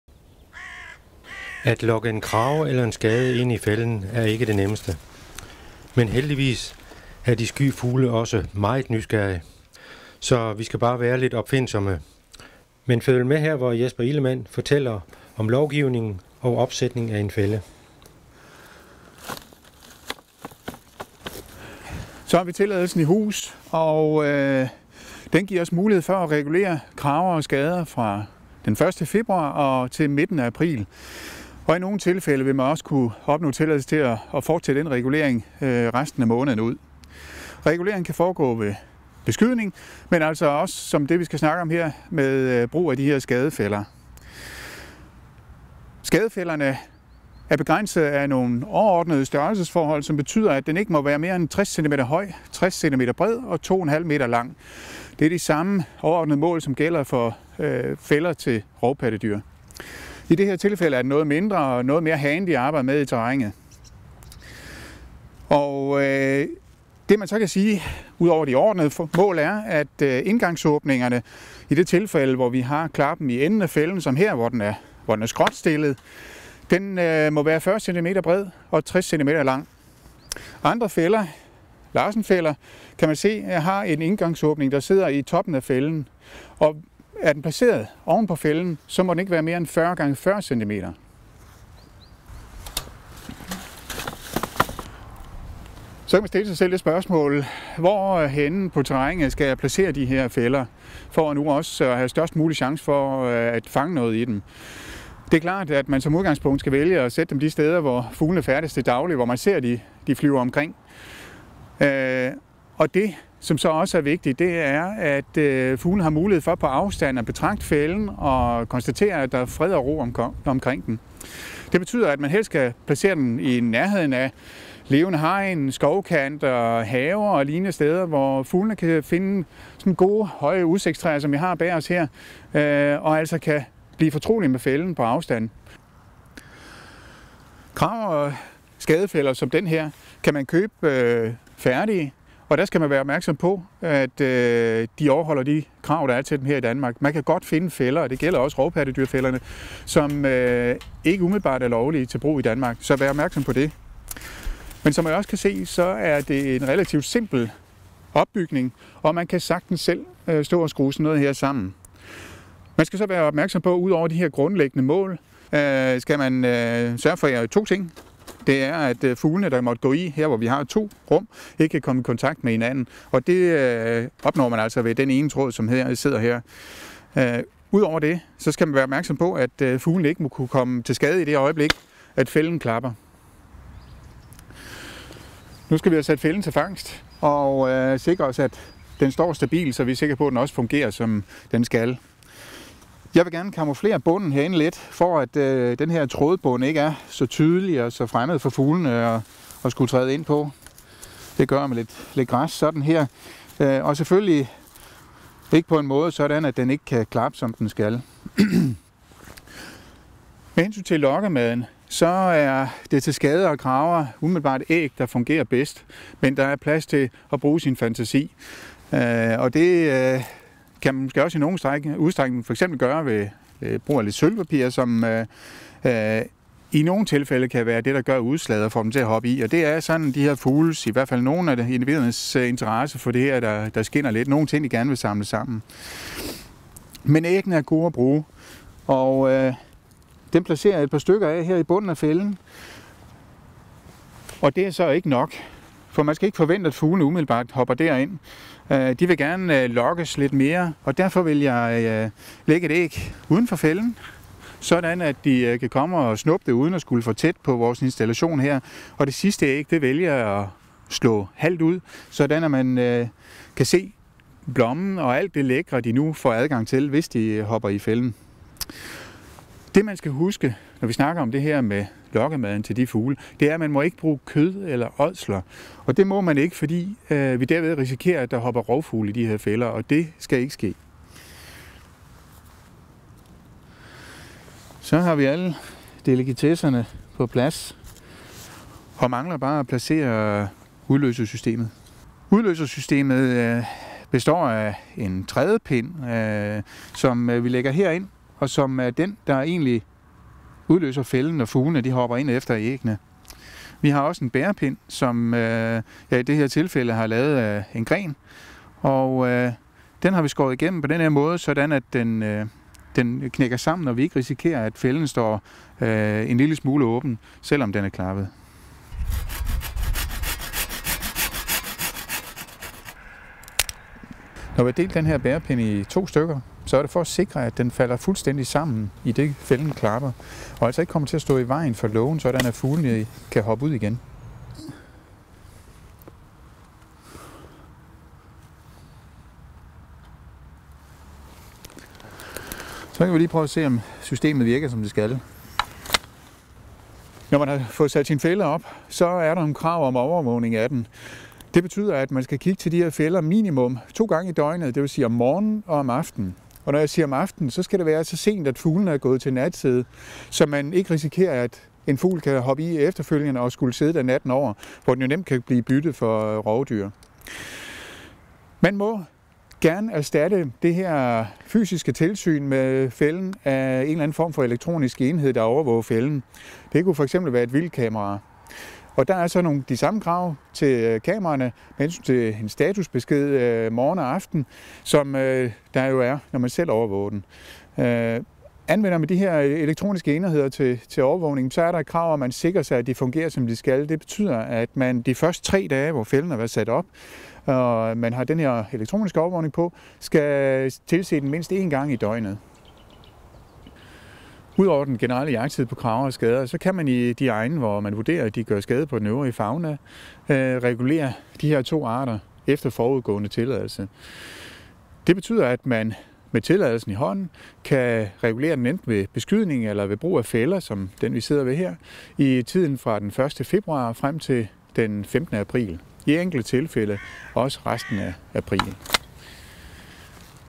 Kragefælder og skadefælder: Regulering af krager og skader i fælder
Du kan regulere skadevoldende vildt ved beskydning, men i visse tilfælde også med fælder. I denne video får du en grundig gennemgang af regler og brug af fælder til regulering af krager og skader.